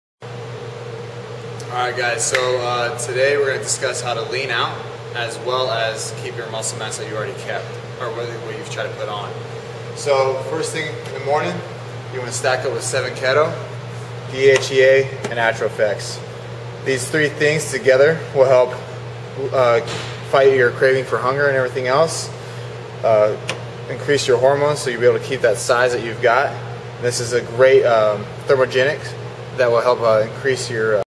AtroPhex by BSN combined with DHEA FOr Sparing Muscle
Ian tells bodybuilders that are dieting how to spare muscle